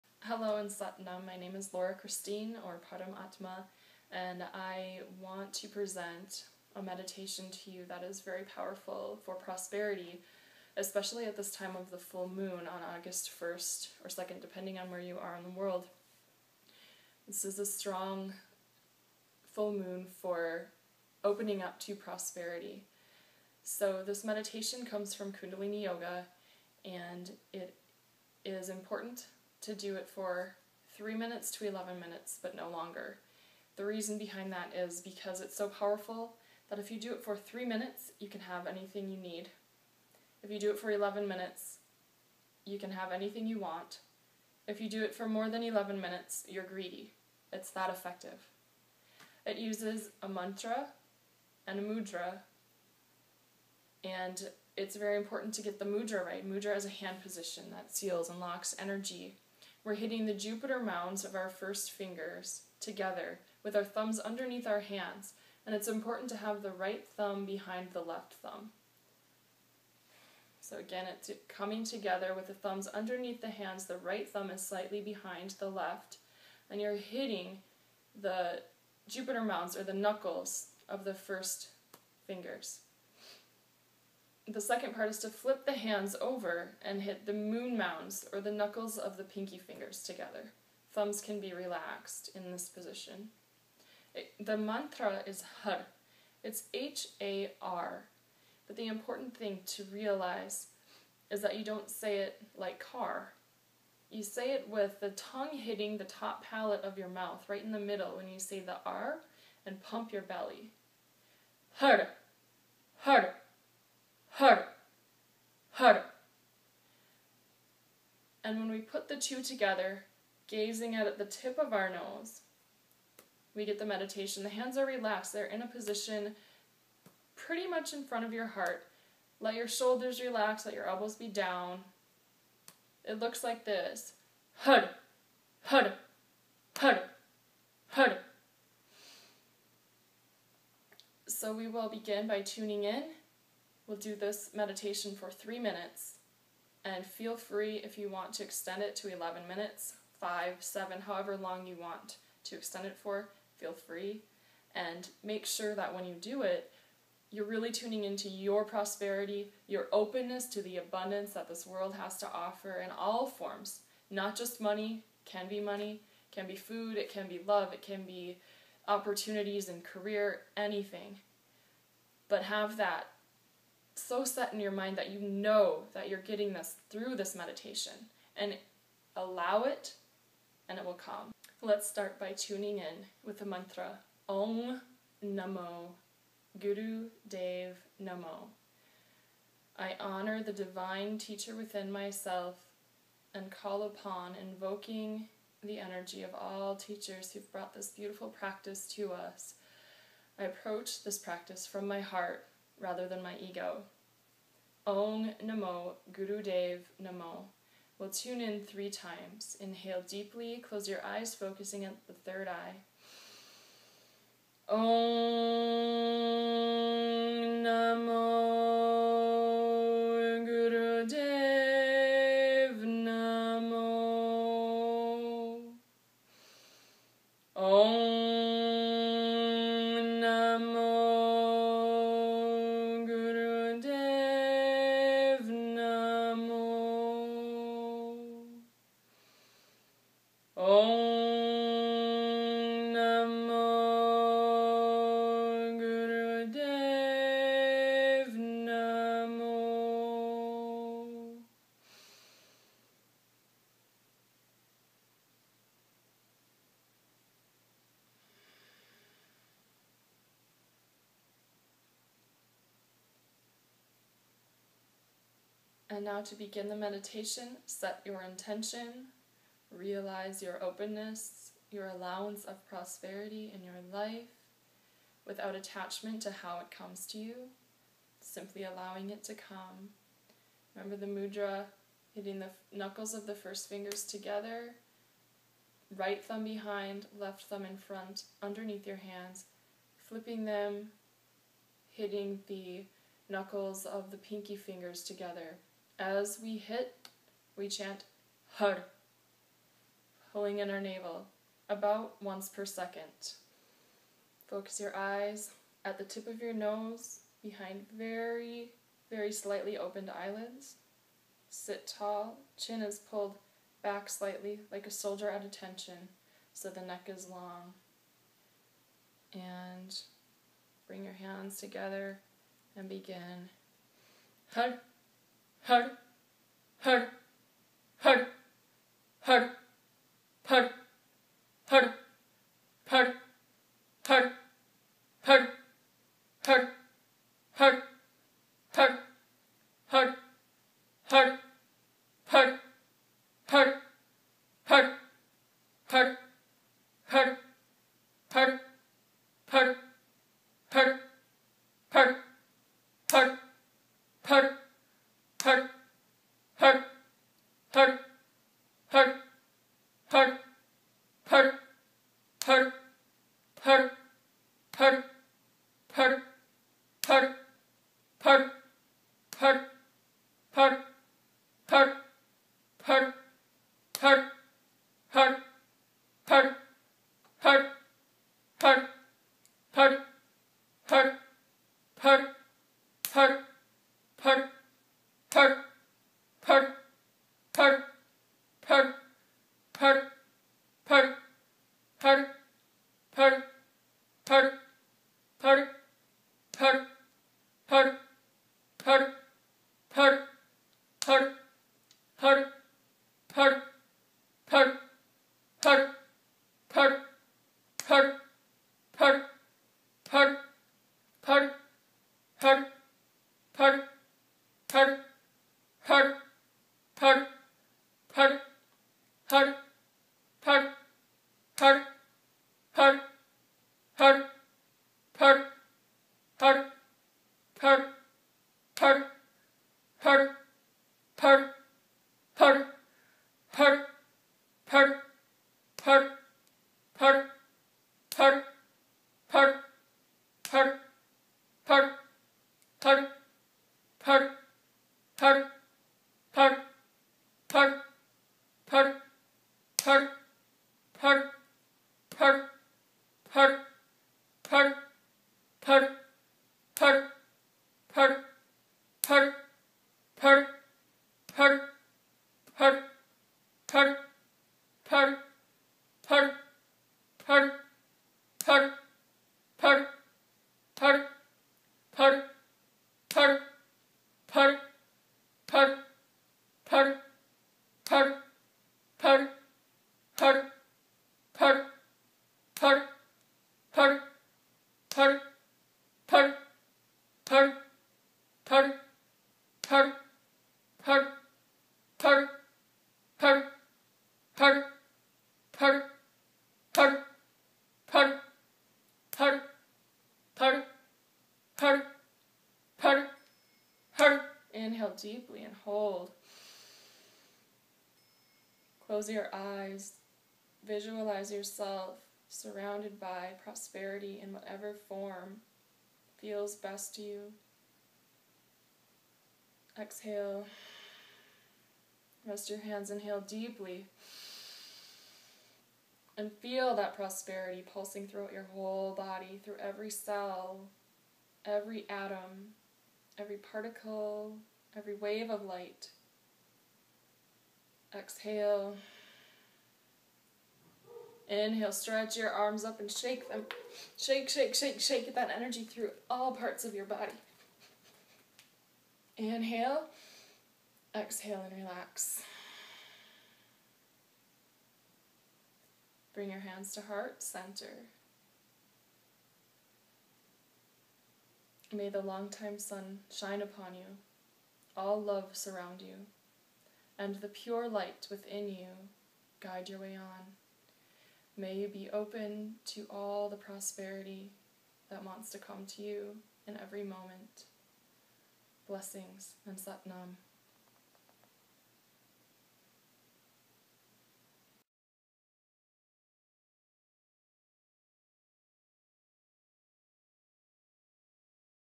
Prosperity Meditation
A quick meditation to support your openness to receiving prosperity in all its forms, from Kundalini Yoga.